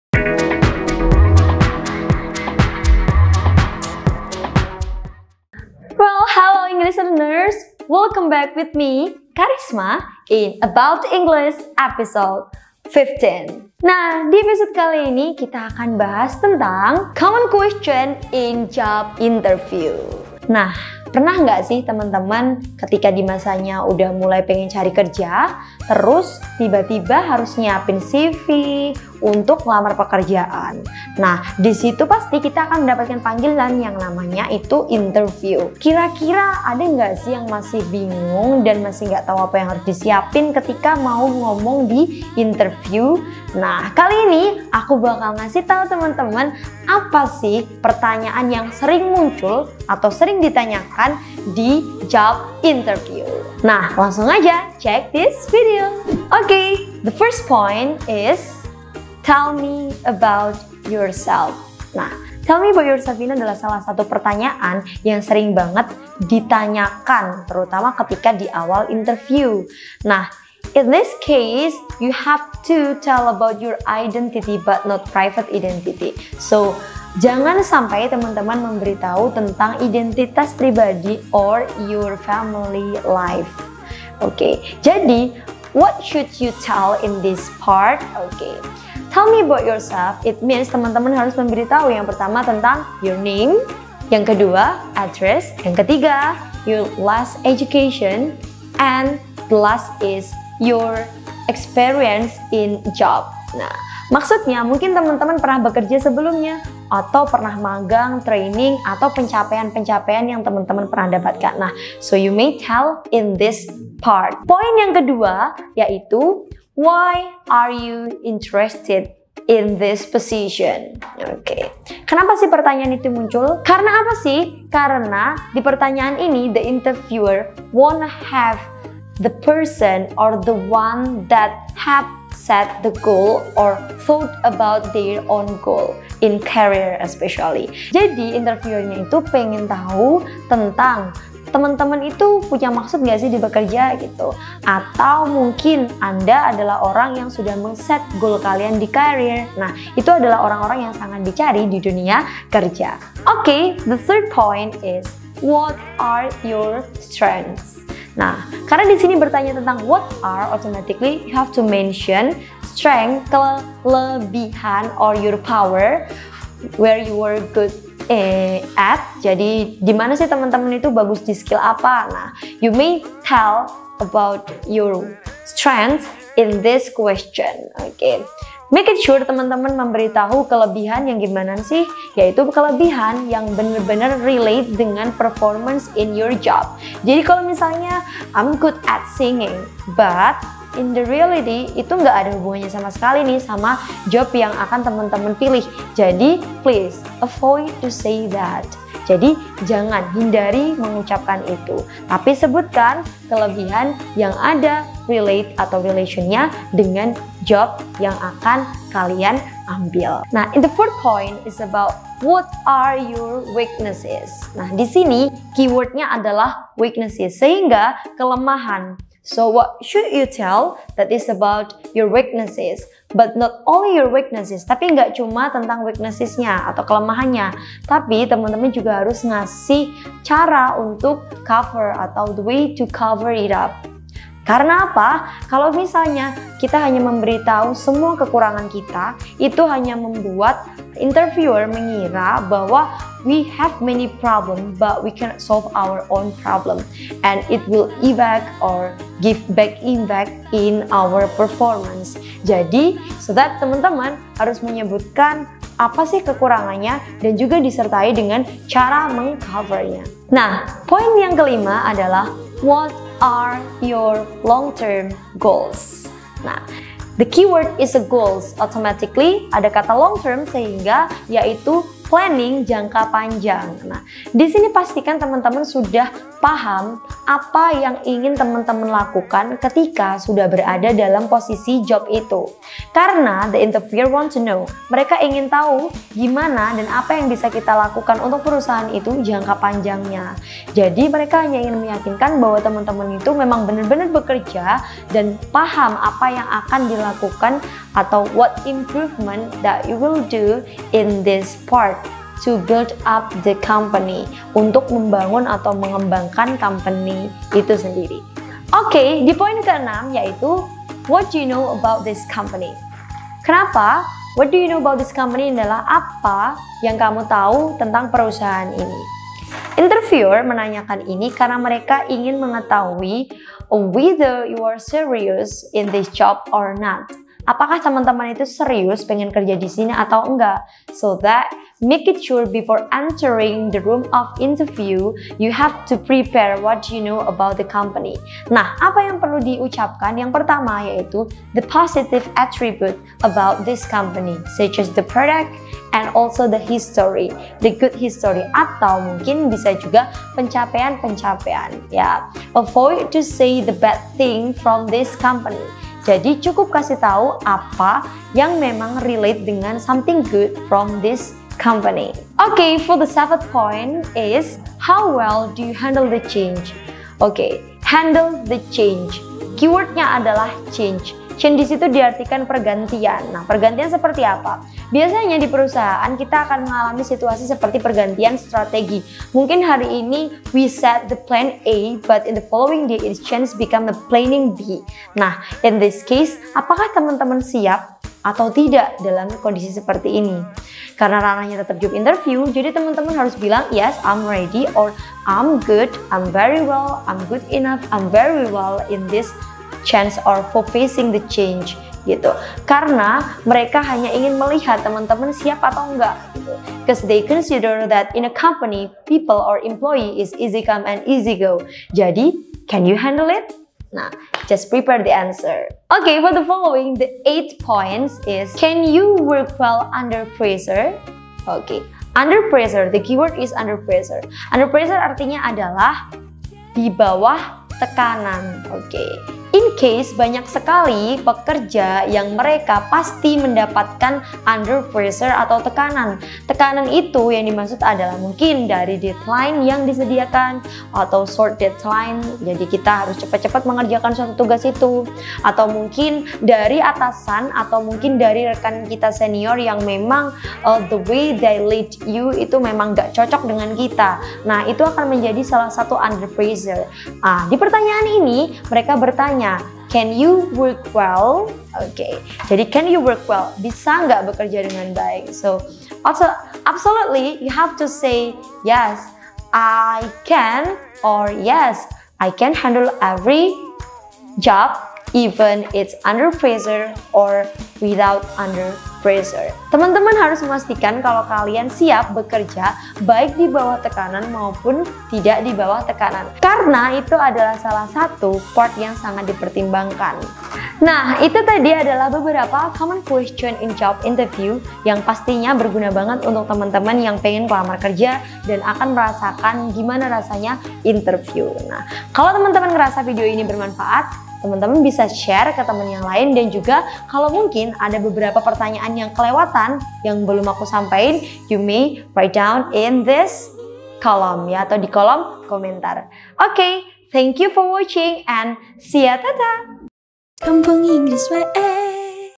Wawancara Kerja Bahasa Inggris dan Jawabannya
Hallo English Learners!
Balik lagi nih di ABout English Eps: 15 di Episode kali ini miss kharisma akan share ke kalian tentang Wawancara Kerja Bahasa Inggris dan Jawabannya.
emang biasanya apa saja sih hal-hal yang harus di perhatikan saat Wawancara Kerja Bahasa Inggris dan Jawabannya?
dalam Wawancara Kerja Bahasa Inggris dan Jawabannya  ada beberapa pertanyaan yang harus di perhatikan diantaranya adalah :
1. Tell me about Yourself?
2. Why are you interested in this position?
3. What your weakness?
4. What are your longterm goals?
5. can you work underpressure?

nah itu beberapa pertanyaan penting saat Wawancara Kerja Bahasa Inggris dan Jawabannya, untuk lebih detail penjelasan detailnya langsung check videonya ya.